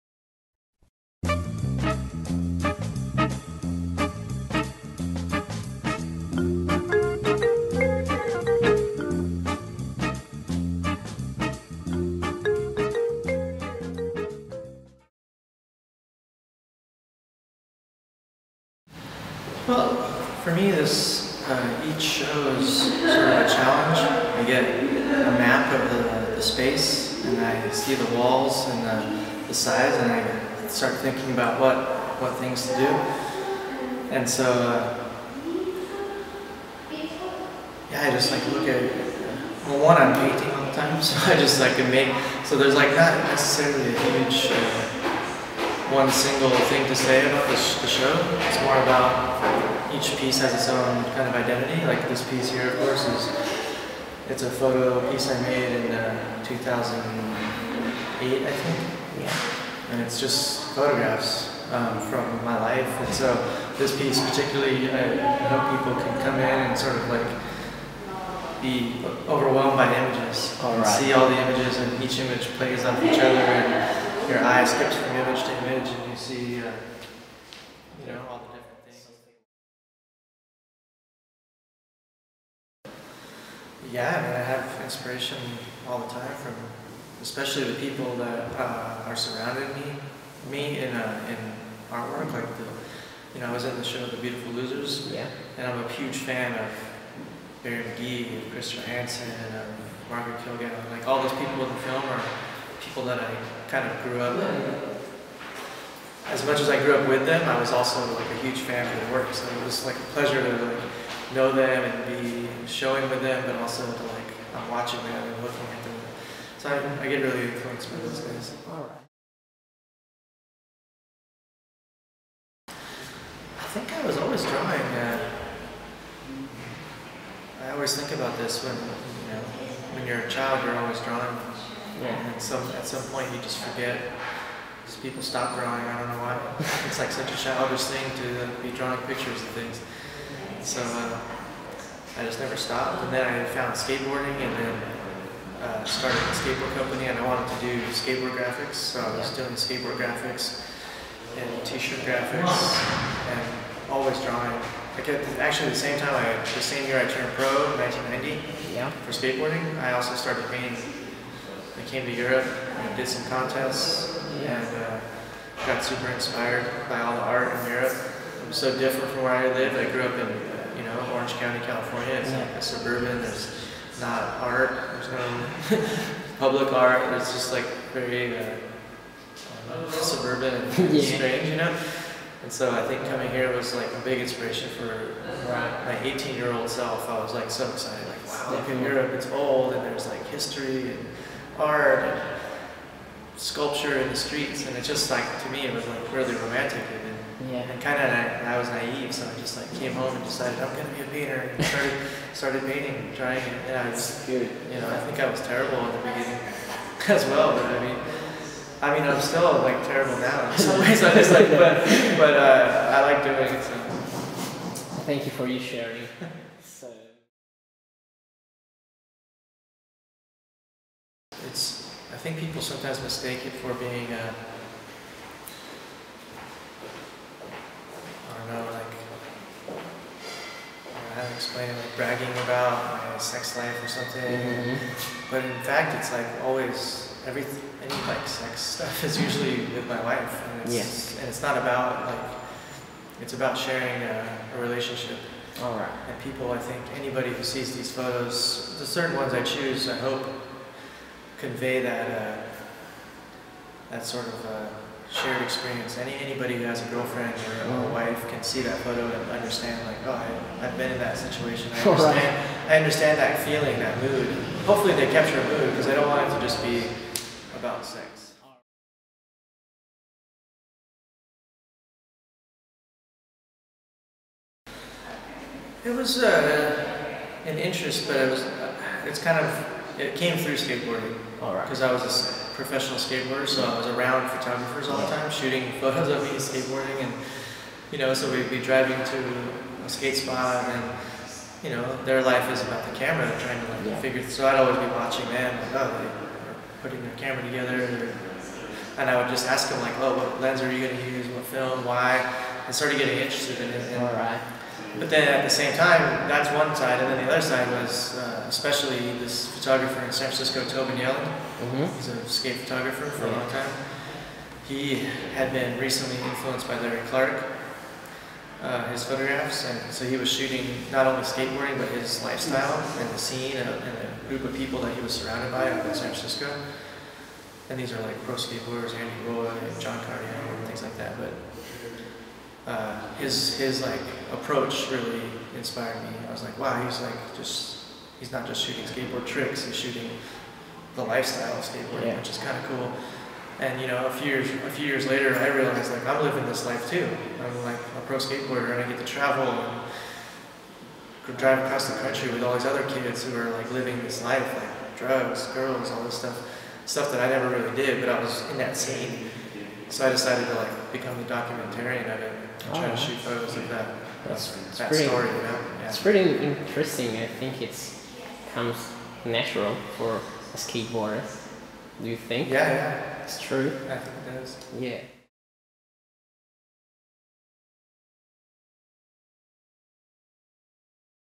A.TV: 5 Questions 2 Ed templeton
Ed Templeton, skateboarder , artist and god father of Toy Machine Skateboards showed up in Italy to inaugurate "a gentle collision". The art show featuring painting, illustrations & photograph produced by the american artist in the last 15 years took place at Jerome Zoo Gallery in Milan. A few day before the show premiered we went over and asked Ed our 5 Questions.  Interview & Video by Ariel Crupi